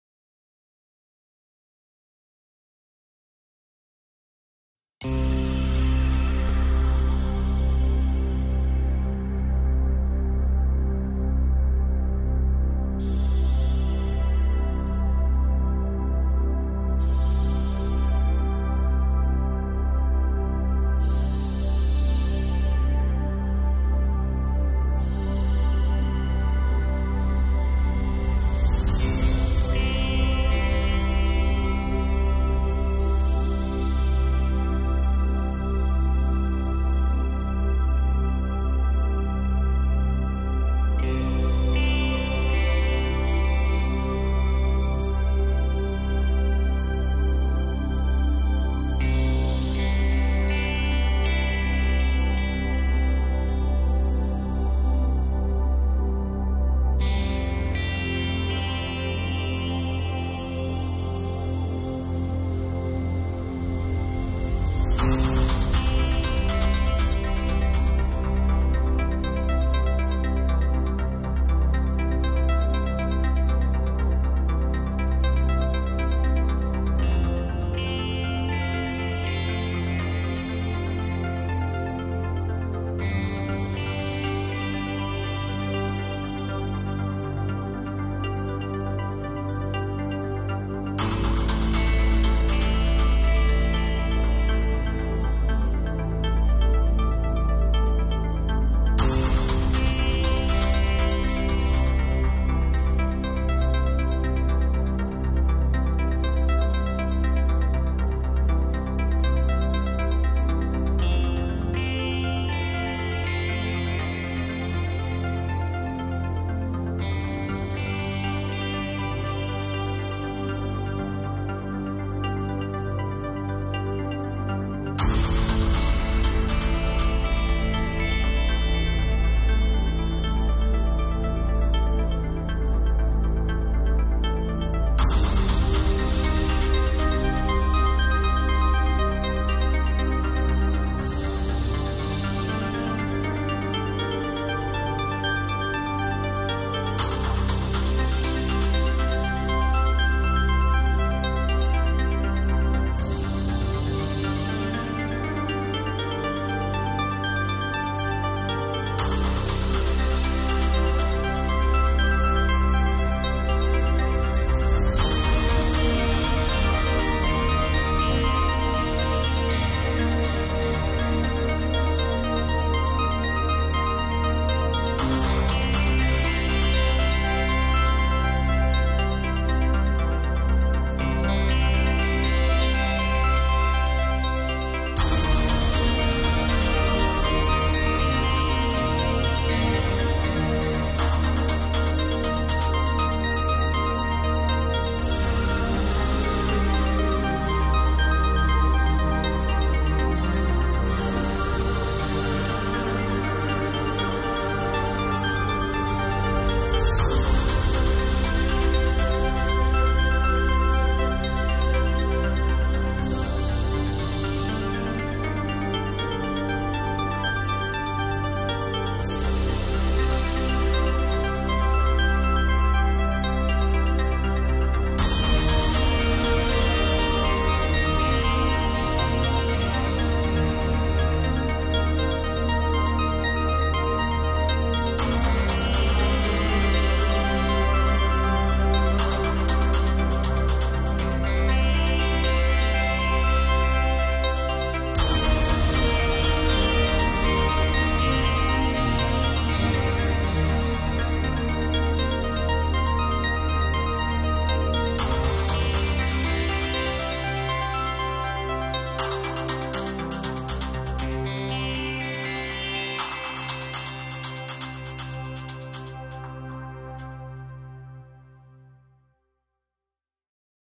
Valles verdes / Green valleys
Lanzo mi dron para que recorra los preciosos valles verdes de los paisajes del jerte y alrededores acompañado de una musica aerea.
I launch my drone to fly over the beautiful green valleys and landscapes of the jerte location and surroundings accompanied by an aerial music.